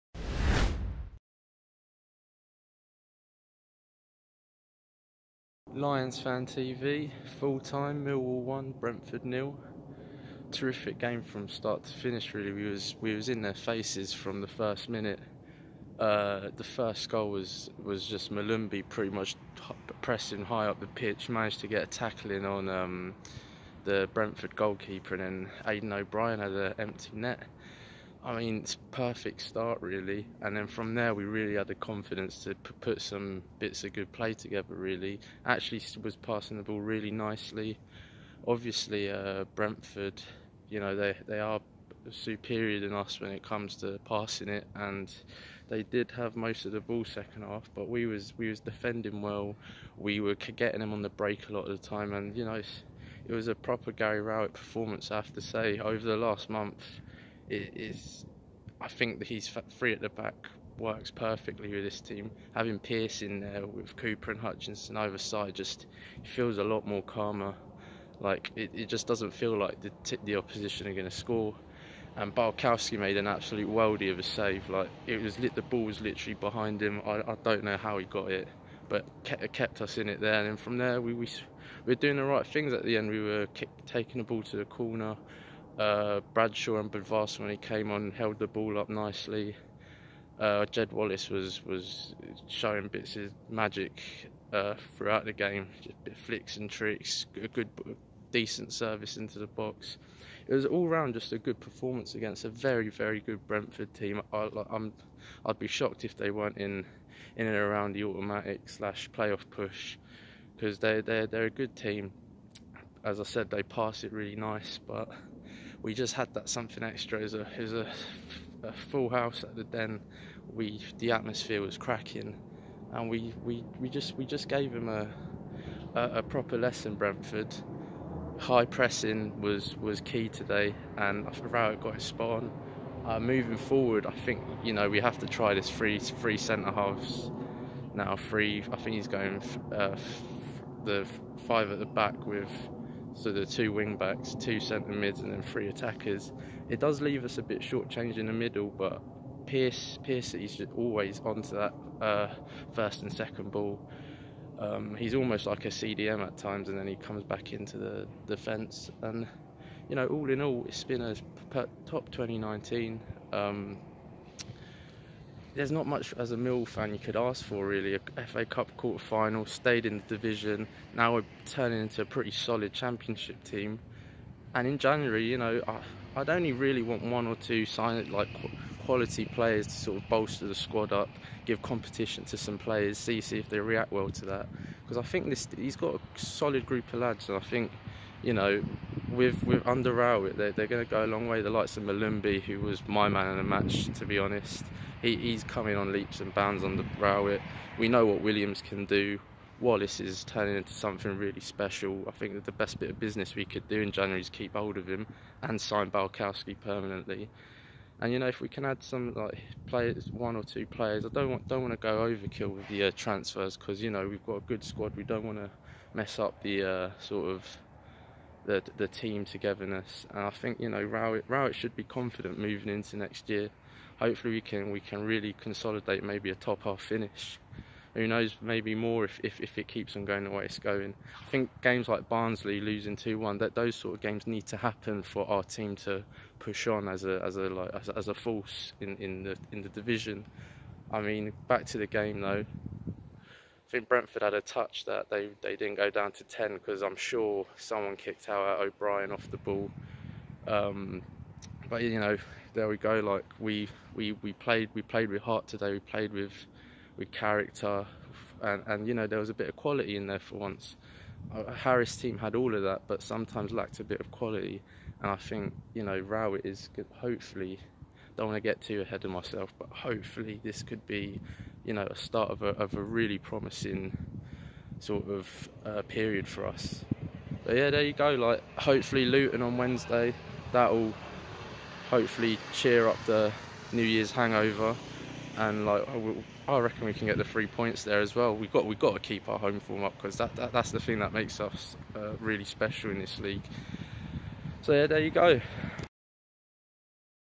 ANTHONYS ANALYSIS- MILLWALL 1-0 BRENTFORD “WE WERE IN THEIR FACES FROM THE FIRST MINUTE”
Fan reaction with Anthony from today’s 1-0 win over Brentford at the Den.